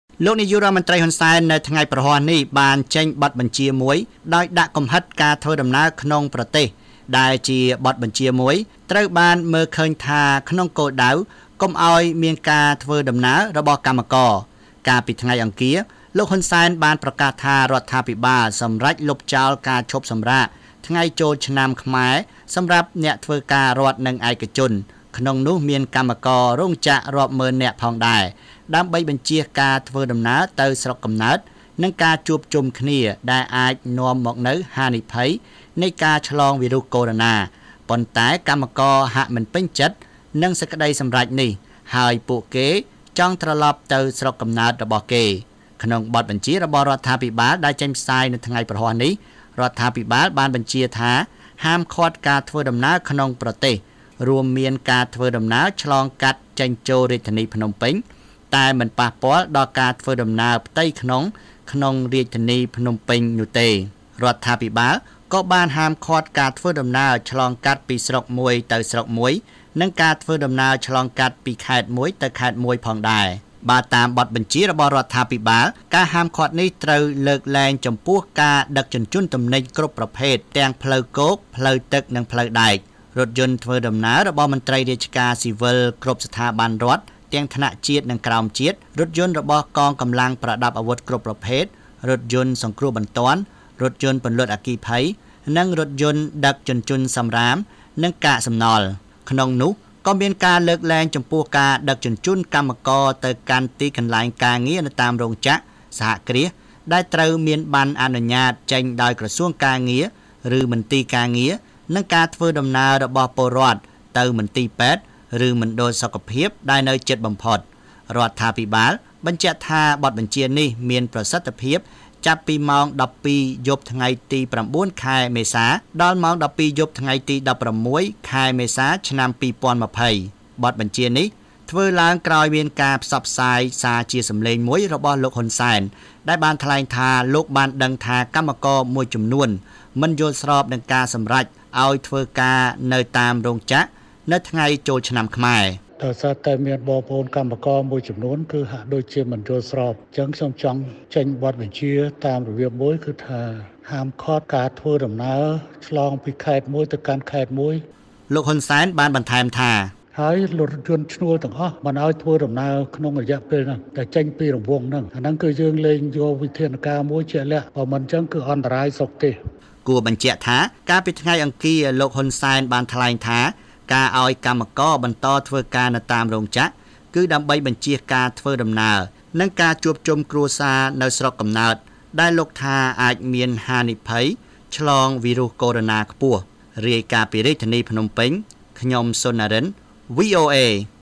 រដ្ឋាភិបាល​បញ្ជា​​បិទ​ការ​ធ្វើ​ដំណើរ​ចាប់​ពីយប់​នេះ​ដល់​​១៦ ខែមេសា
លោក​ ហ៊ុន សែន​ នៅ​ថ្ងៃ​នេះ ​បាន​ចេញ​បទ​បញ្ជា​មួយ​បិទ​ការ​ធ្វើ​ដំណើរ​ពី​ភ្នំពេញ​ទៅ​តាម​បណ្តា​ខេត្ត​ និង​ស្រុក​ផង​ដែរ។ ​ការ​បិទ​នេះ​ចូល​ជា​ធរមាន​ចាប់​ពី​យប់​ថ្ងៃ​ទី​៩ ​ខែ​មេសា​នេះ​ដល់​ថ្ងៃ​ទី១៦​ខែមេសា។ ​កាលពី​ថ្ងៃអង្គារ​ លោក​នាយក​រដ្ឋមន្រ្តី ហ៊ុន សែន​បាន​សម្រេច​ផ្អាក​ការ​ឈប់​សម្រាក​ចូល​ឆ្នាំ​ខ្មែរ​ និង​ឲ្យ​កម្មករ​រោងចក្រ​ធ្វើការ​ធម្មតា​ដើម្បី​បញ្ចៀស​លទ្ធភាព​ឆ្លង​វីរុស​កូរ៉ូណា​នៅ​តាម​បណ្តា​ខេត្ត។​ ការណ៍​នេះនាំ​ឲ្យ​កម្មករ​មួយ​ចំនួន​មិន​បំពេញ​ចិត្ត។លោក ស៊ុន ណារិន នៃ​វីអូអេ រាយការណ៍​លម្អិត​ពី​ភ្នំពេញ។